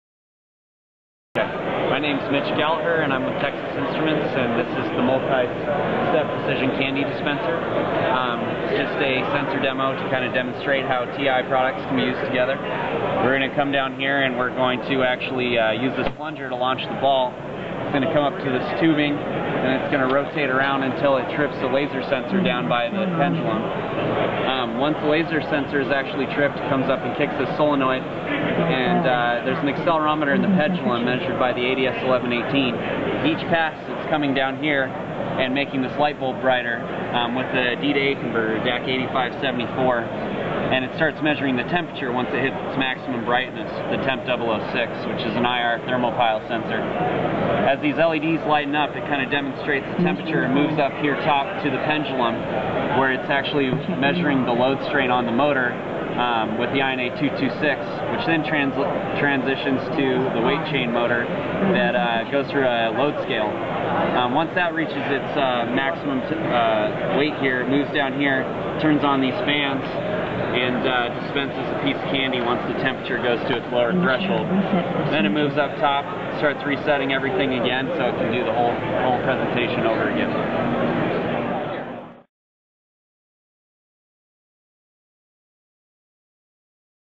Like Taking Candy from a Machine
Mitch Gallagher from Texas Instruments explains the function of the company's candy dispenser demo at Sensors Expo 2012 and the role that various products play to make it work. The display incorporates seven of TI's products including the TMP006 MEMS IR temperature sensor.